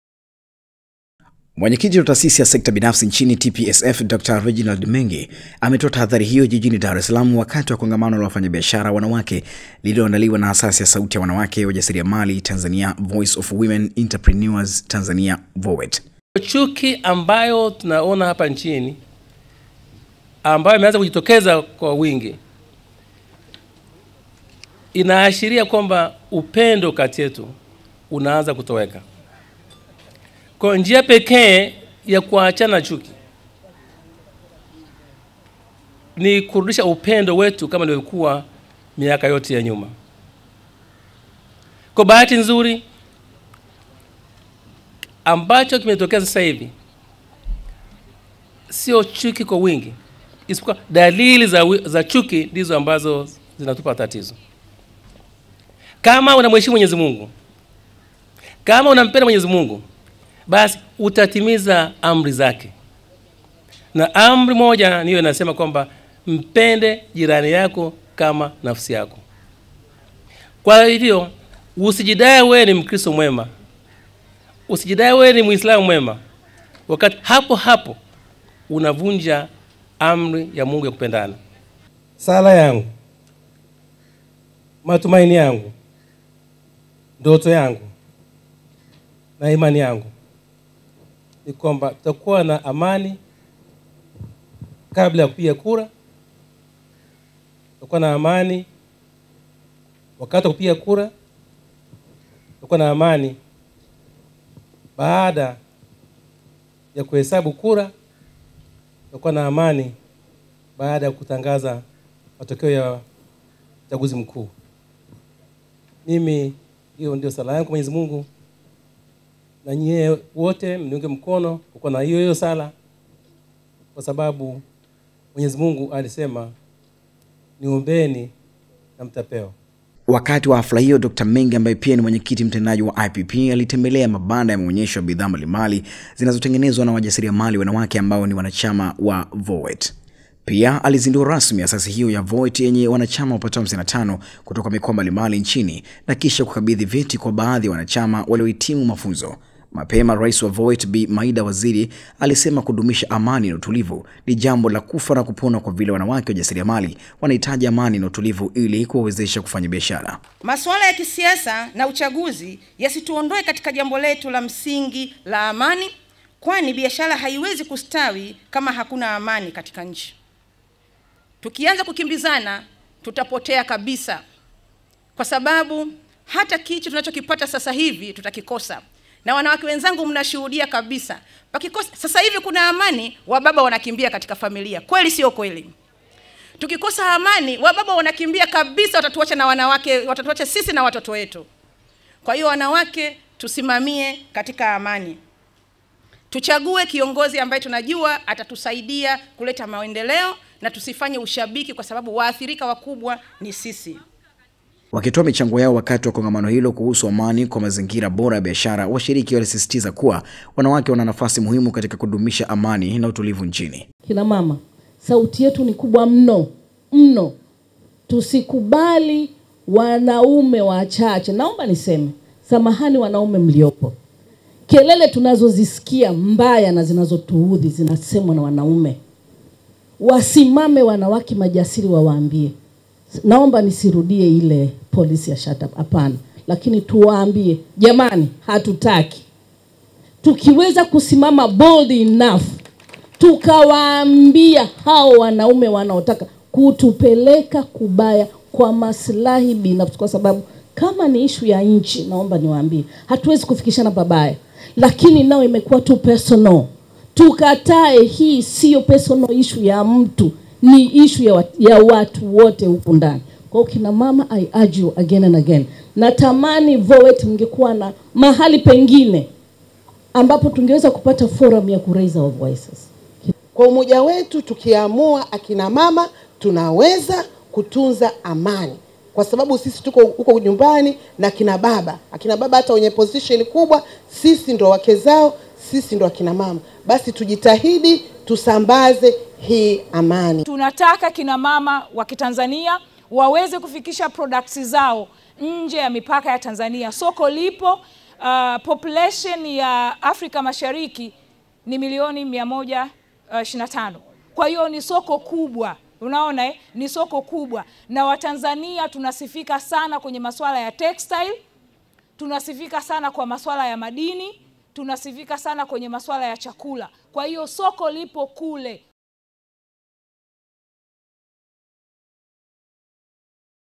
Dr Reginald Mengi azindua asasi ya sauti ya wanawake wajasiriamali Tanzania.
Juhudi za maksudi zinahitajika kukomesha kansa ya chuki na uongo inayoanza kujitokeza nchini, ili kudumisha amani na upendo wakati huu wa kuelekea uchaguzi mkuu.